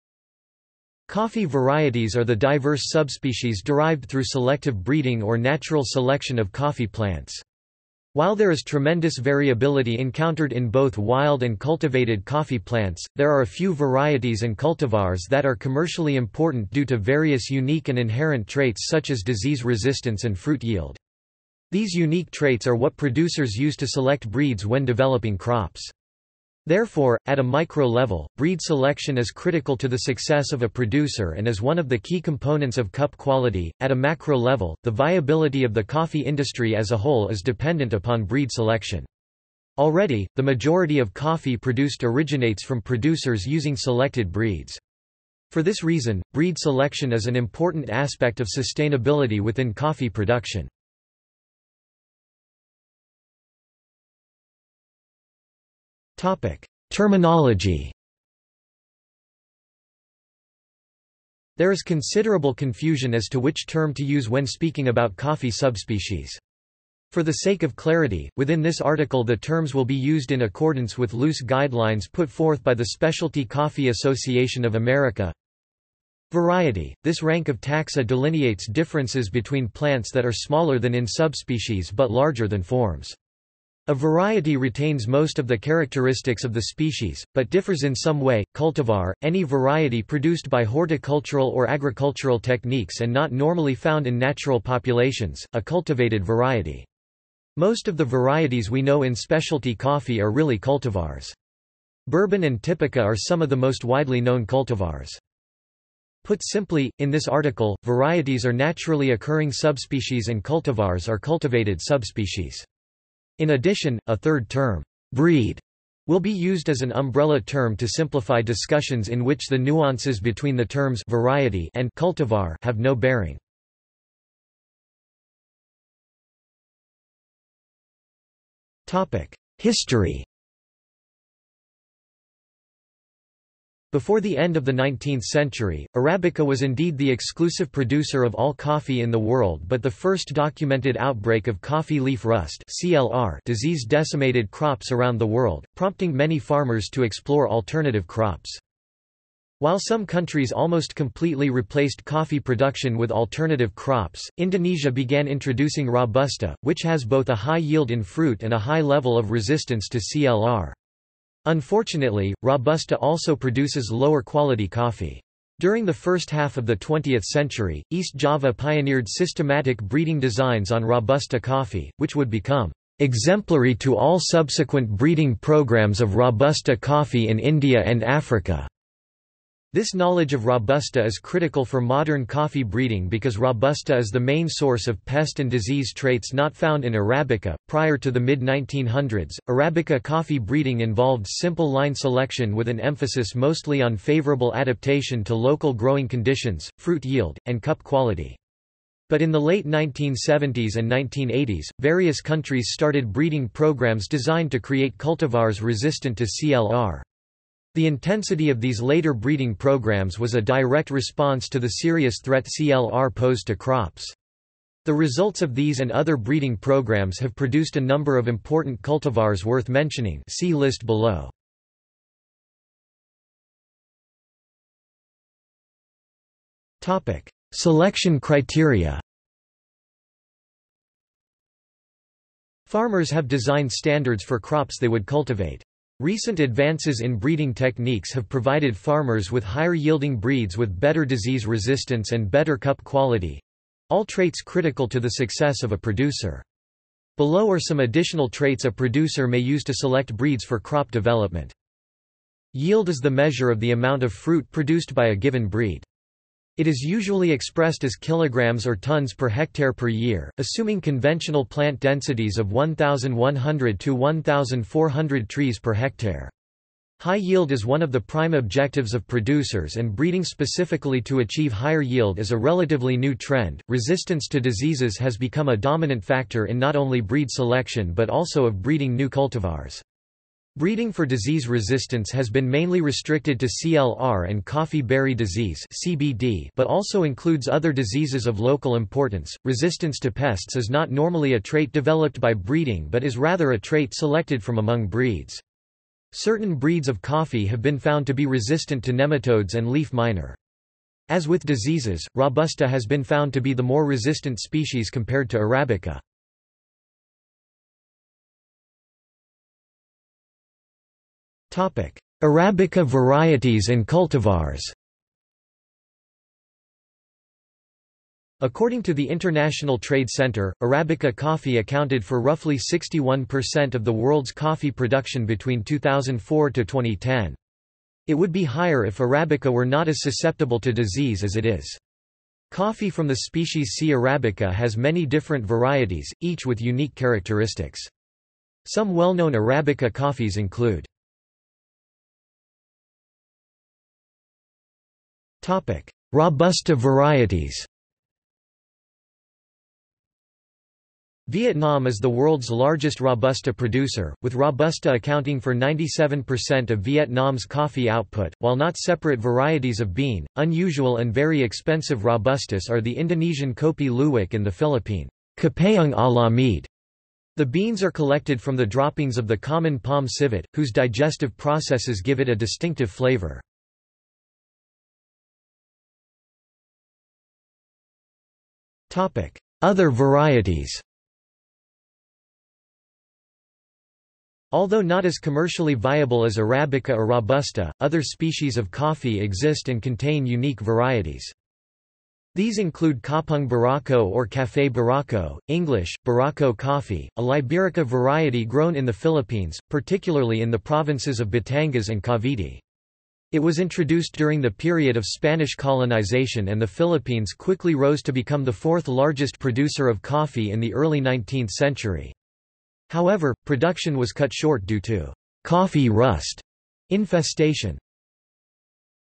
List of coffee varieties | Wikipedia audio article
This is an audio version of the Wikipedia Article:
List of coffee varieties

00:00:52 1 Terminology
00:02:05 2 History
00:03:43 3 Selection criteria
00:05:07 4 Arabica varieties and cultivars
00:05:41 5 Robusta varieties
00:06:15 6 Other varieties

- Socrates

SUMMARY
Coffee varieties are the diverse subspecies derived through selective breeding or natural selection of coffee plants.  While there is tremendous variability encountered in both wild and cultivated coffee plants, there are a few varieties and cultivars that are commercially important due to various unique and inherent traits such as disease resistance and fruit yield.   These unique traits are what producers use to select breeds when developing crops.  Therefore, at a micro level, breed selection is critical to the success of a producer and is one of the key components of cup quality.At a macro level, the viability of the coffee industry as a whole is dependent upon breed selection.  Already, the majority of coffee produced originates from producers using selected breeds.  For this reason, breed selection is an important aspect of sustainability within coffee production.